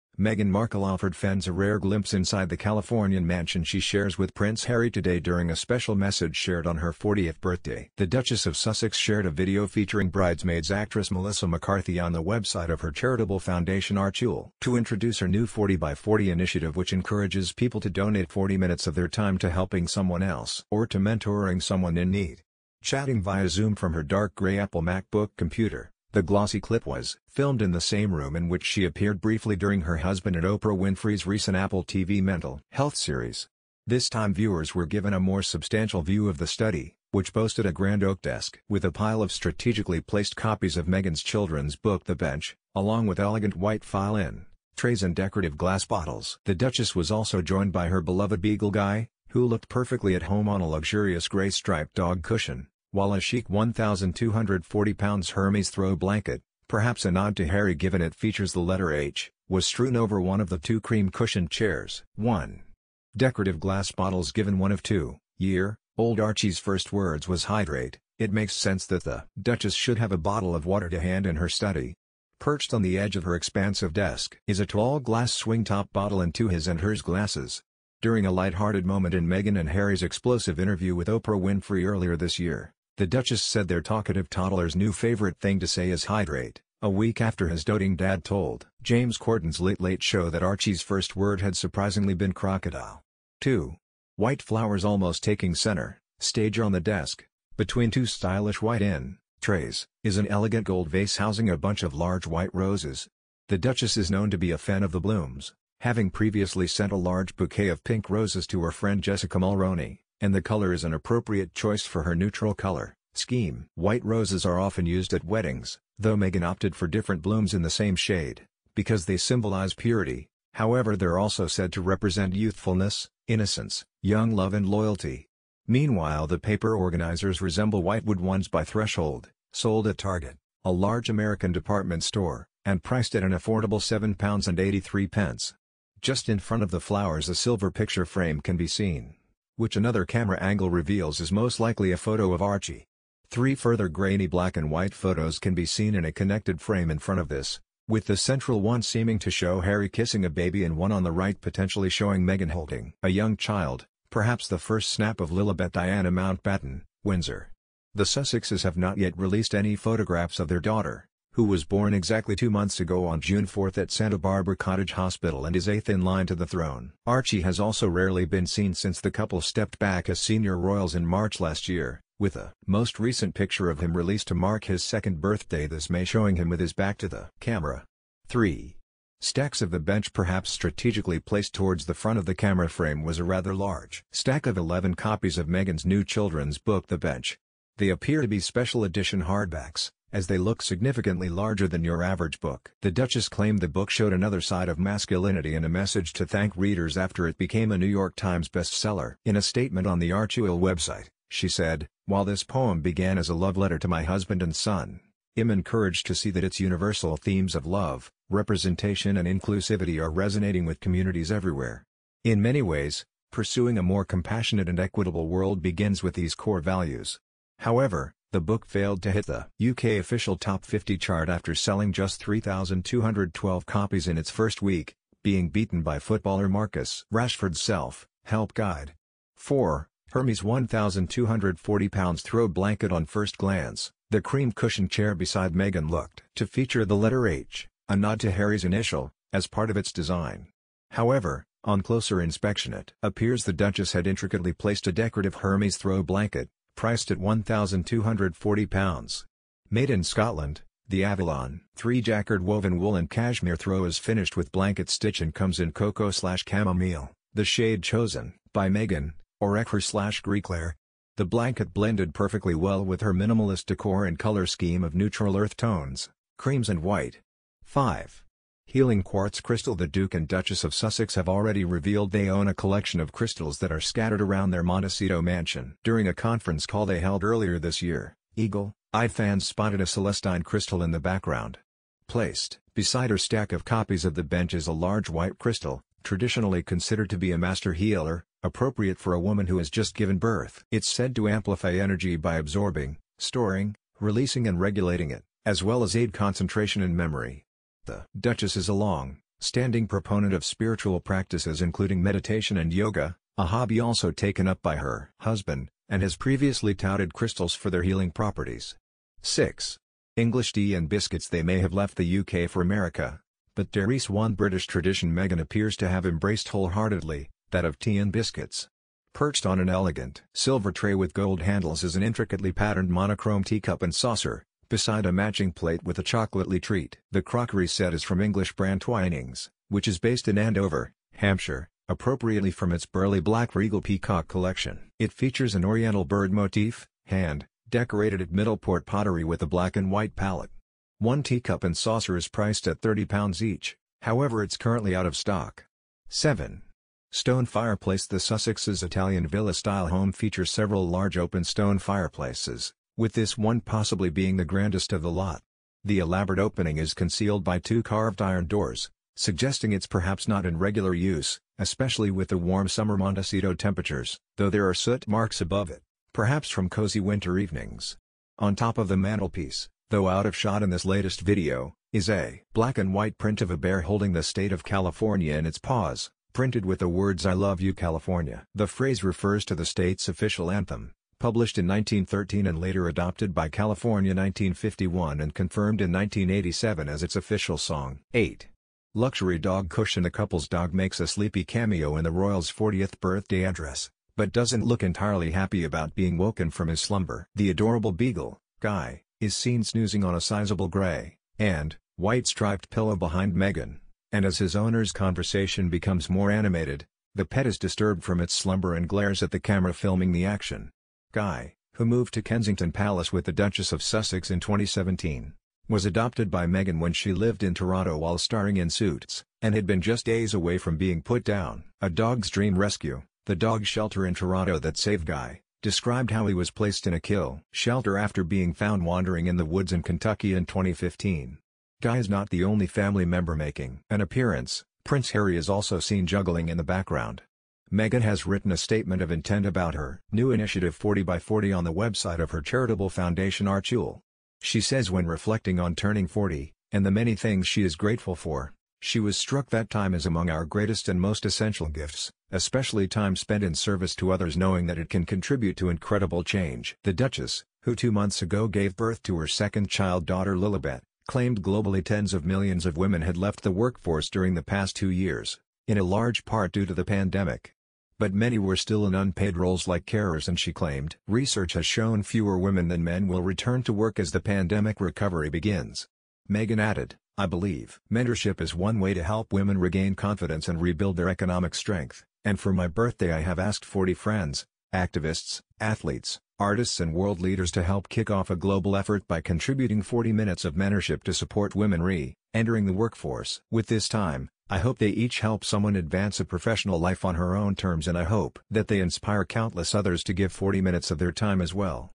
Inside Meghan Markle's California-chic study: Duchess's desk features a healing quartz crystal, En
Meghan Markle offered fans a rare glimpse inside the Californian mansion she shares with Prince Harry today during a 'special message' shared on her 40th birthday. The Duchess of Sussex shared a video featuring Bridesmai